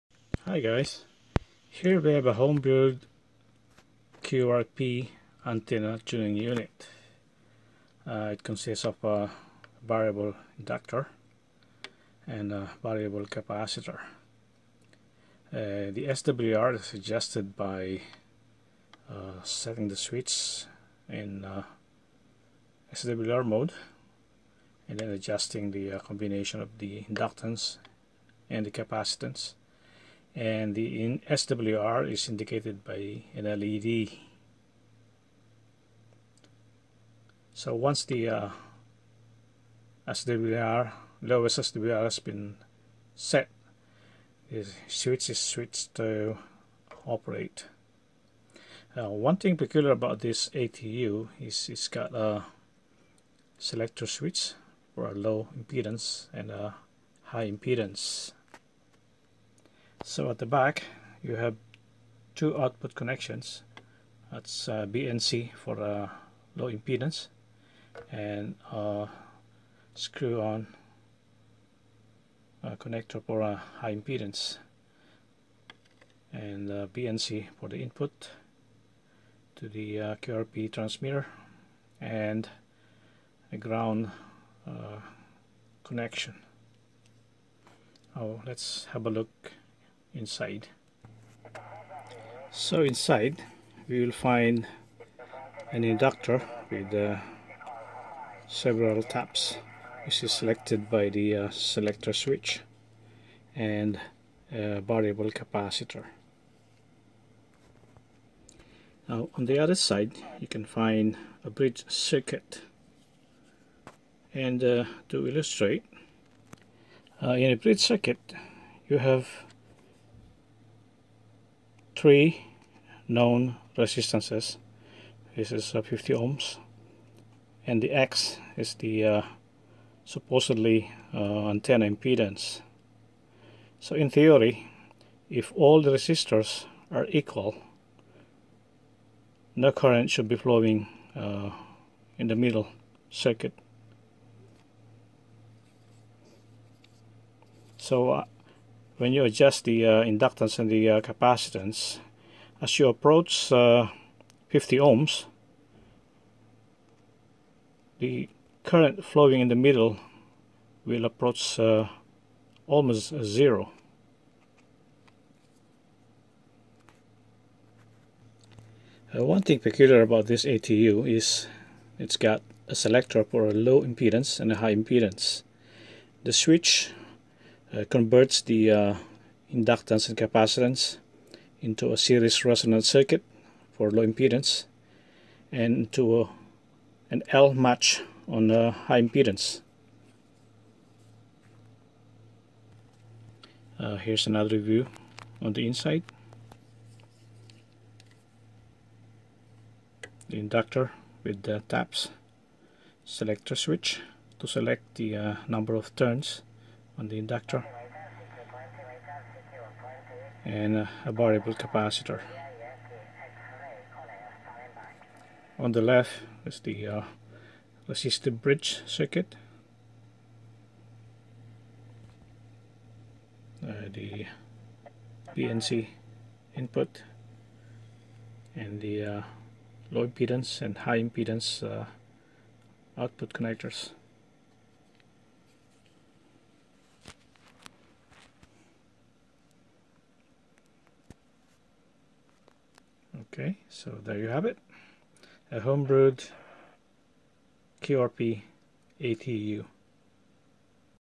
QRP ATU Antenna Tuning Unit - Homebrew Ham Radio
A quick look at a homebrew ATU with LED swr
indicator.